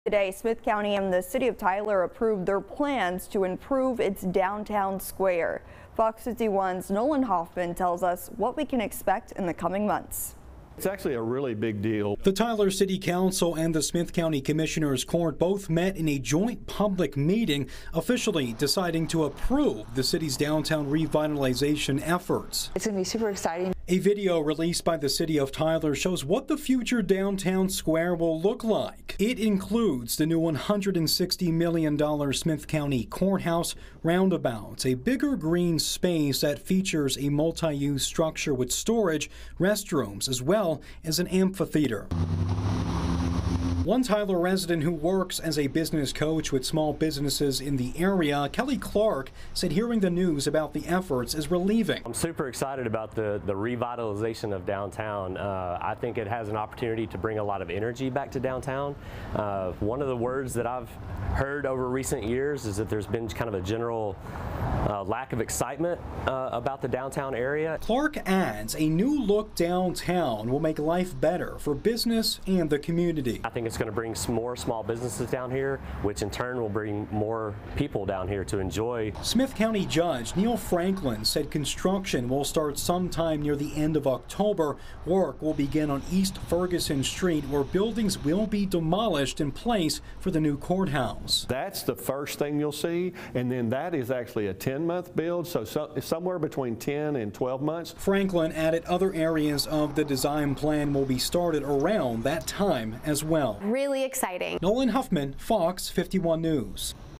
New downtown Tyler design plans approved in special meeting by city council, county commissioners co
FOX51 NEWS AT 9:00 PM